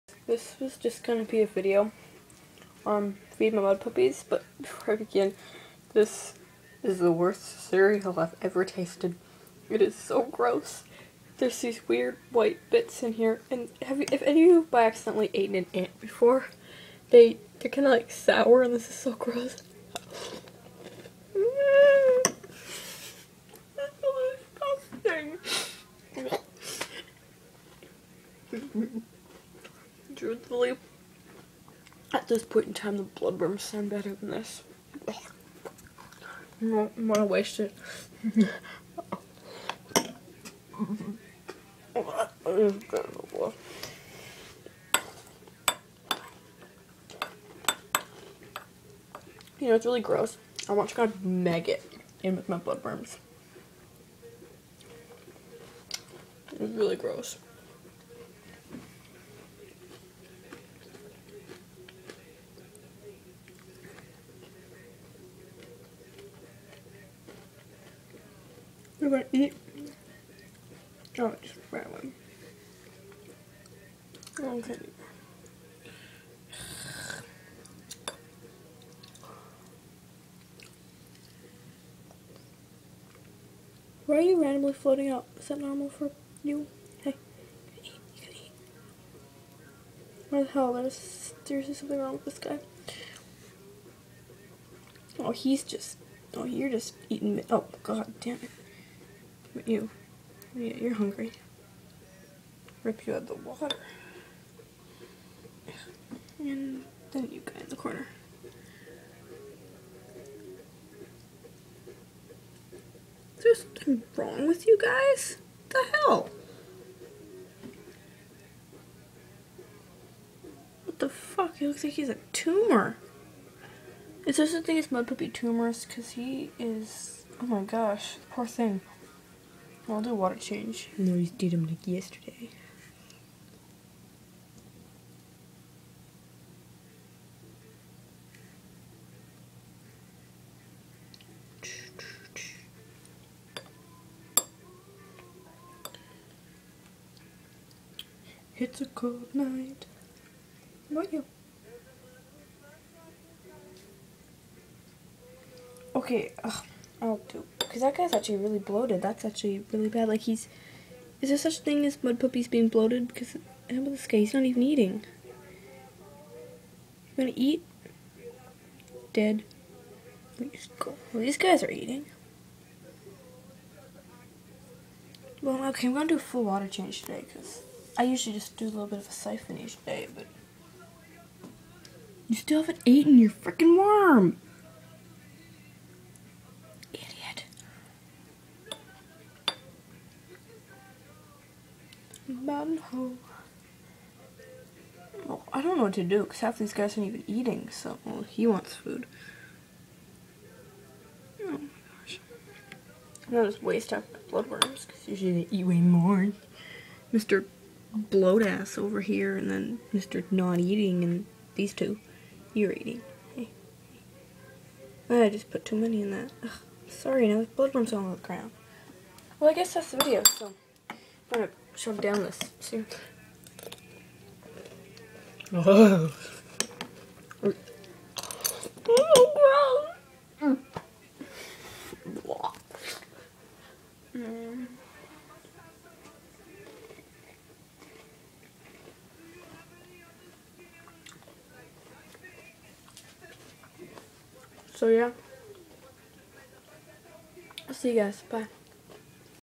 Disgusting cereal/ mudpuppy feeding time!
So it's 12:00 and I never did finish the cereal I dumped it and got fruit loops. Also I'm sad my mudpuppy has some sort of bloat and the other is just not eating (they were all fine yesterday) I do around 10-25% water changes daily so ya :( anyhow. I should go to bed :P